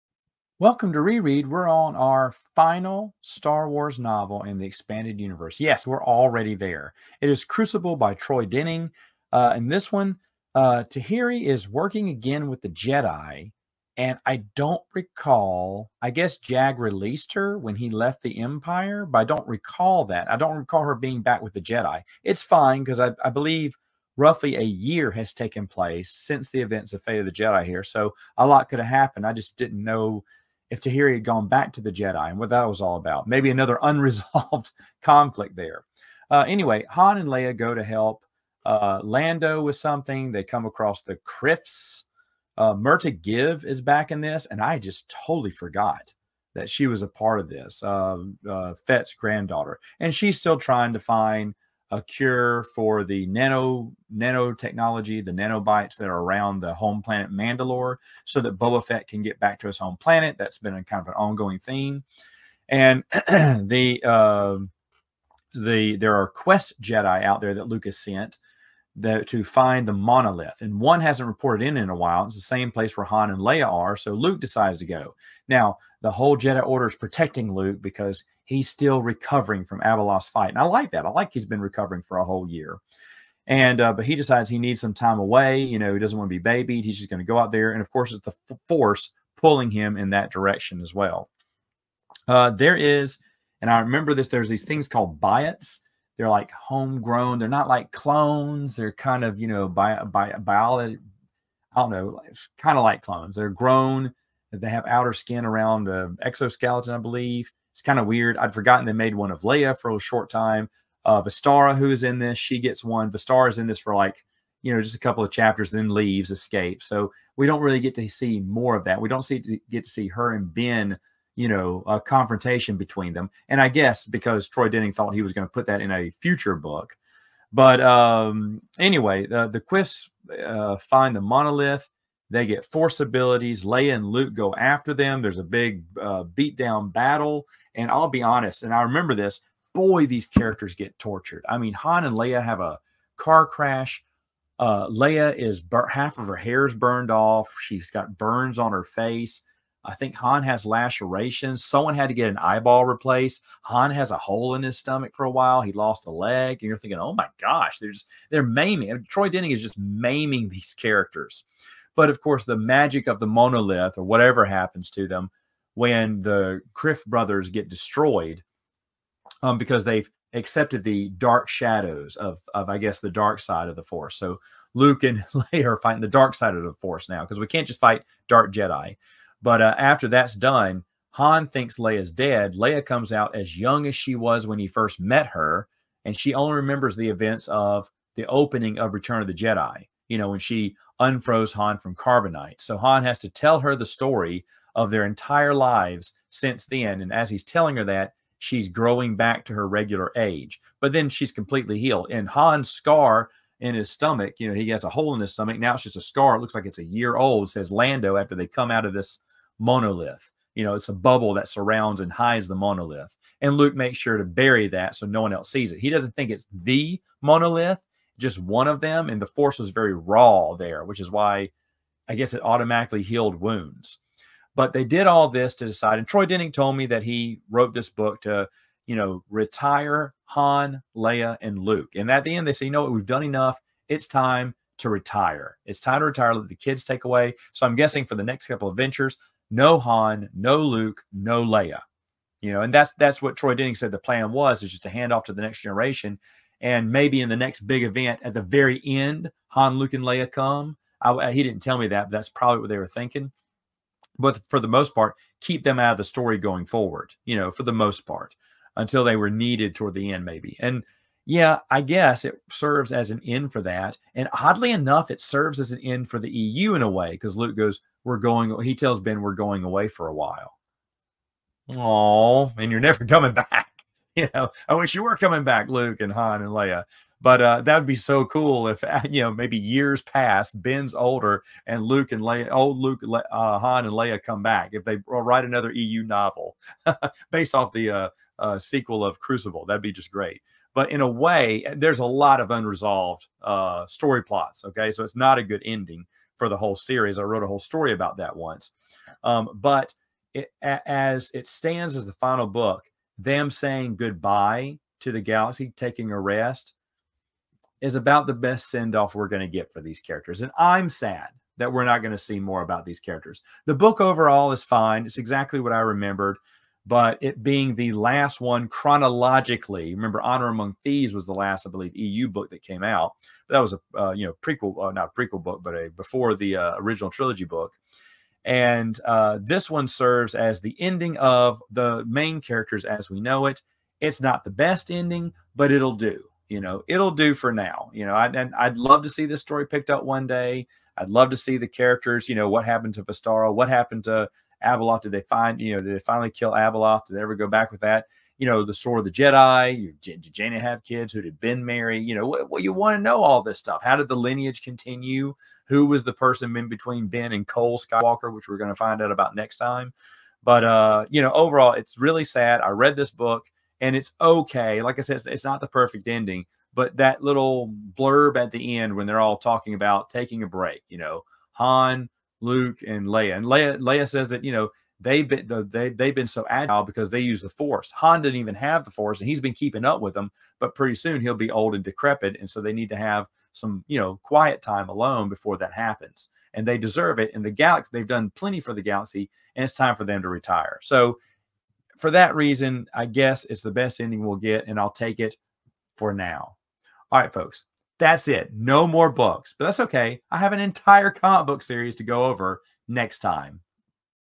Re-Read: Crucible
Today I reread the final book in the EU timeline.